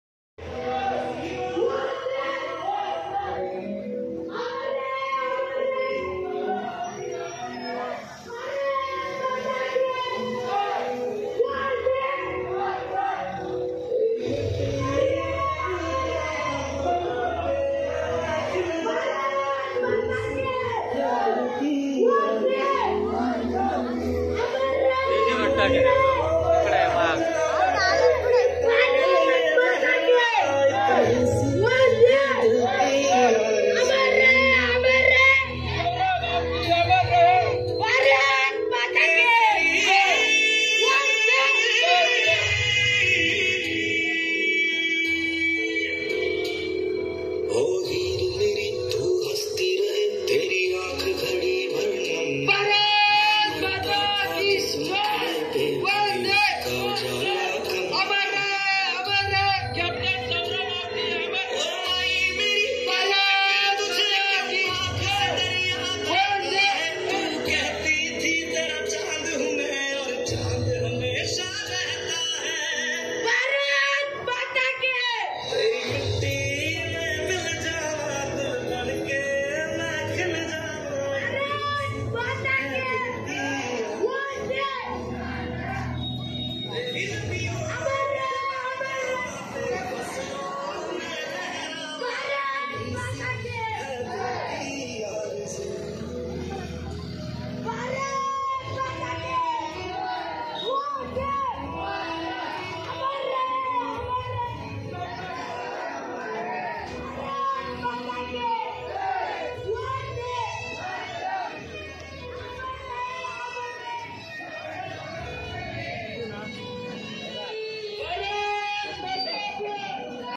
Indian Army | शहीद कॅप्टन सौरभ औटी यांच्यावर शासकीय इतमामात अंत्यसंस्कार | Maharashtra Times
राळेगणसिद्धी येथील लेफ्टनंट सौरभ औटी यांचे जम्मू- काश्मीरमधील लडाख भागात कर्तव्यावर असताना अपघाती निधन झाले.
त्यांच्यावर राळेगणसिद्धी येथे सोमवारी सकाळी लष्करी इतमामात अंत्यसंस्कार करण्यात आले.
लेफ्टनंट औटी कर्तव्यावर असताना मागील आठवड्यात त्यांच्या वाहनाला अपघात झाला होता.
त्यात ते गंभीर जखमी झाले होते. त्याच्यावर उपचार चालू असताना शनिवारी त्यांचे निधन झाले.
राळेगणसिद्धीमध्ये साश्रूनयनांनी त्यांना अखेरचा निरोप देण्यात आला.